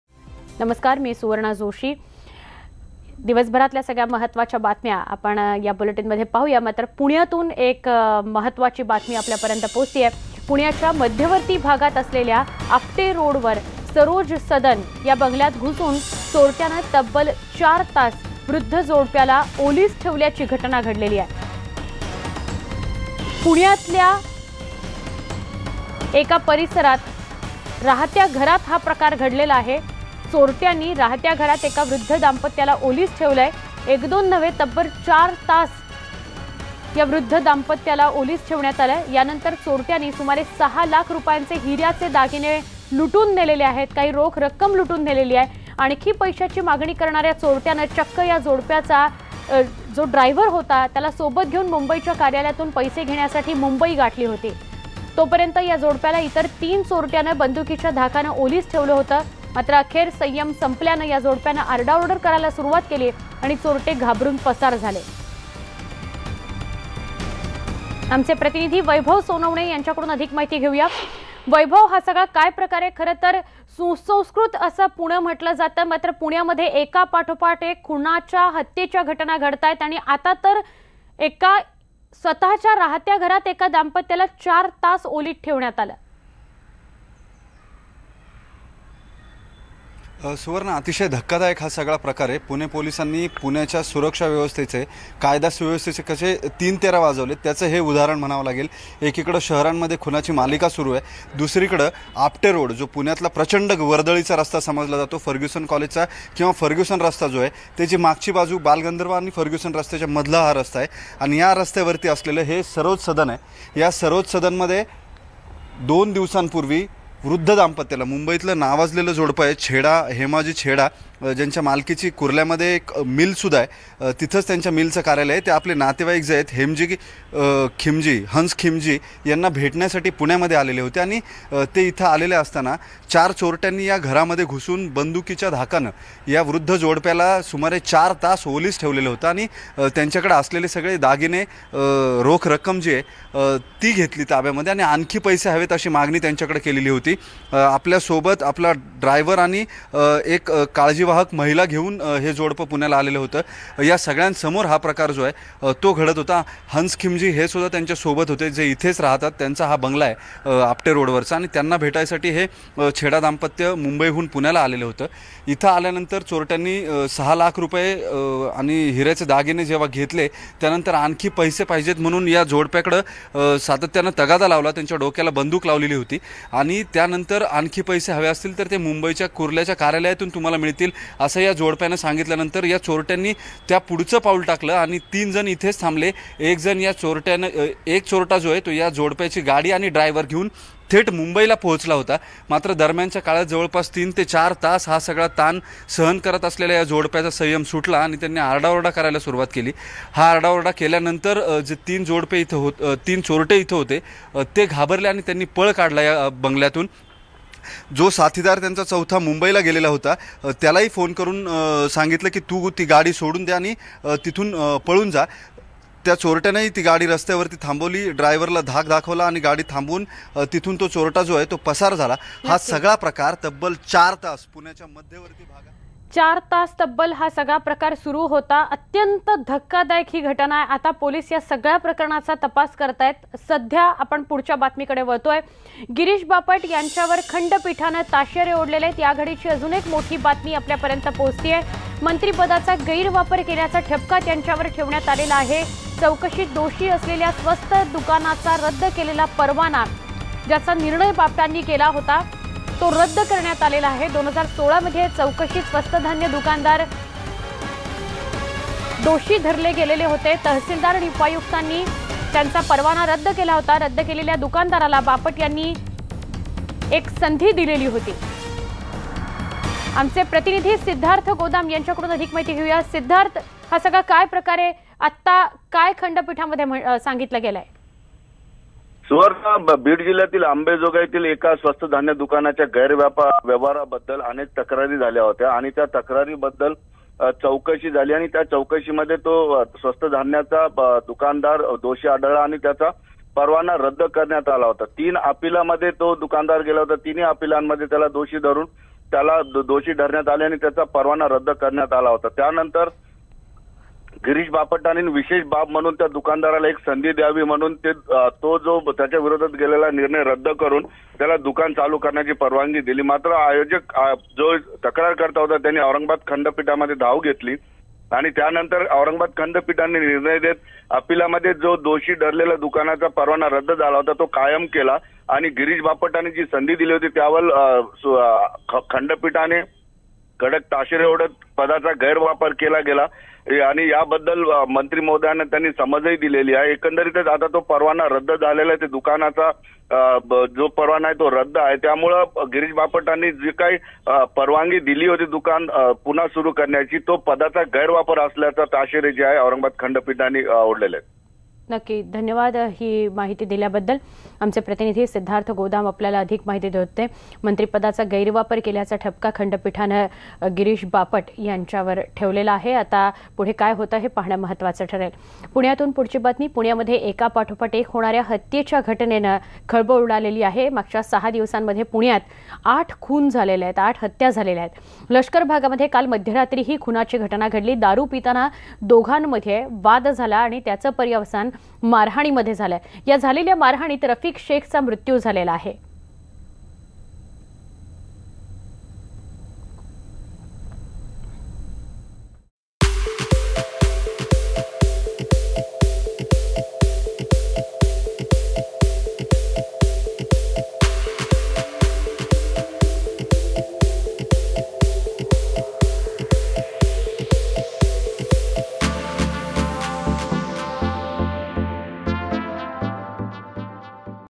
पुण्याच्या मध्यवर्ती आपटे रोड भागातली धक्कादायक घटना | 18 JAN 2019 | NEWS18 LOKMAT
पुण्याच्या मध्यवर्ती आपटे रोड भागातली धक्कादायक घटना,तब्बल ४ तास वृद्ध दाम्पत्याला ठेवलं होतं ओलीस.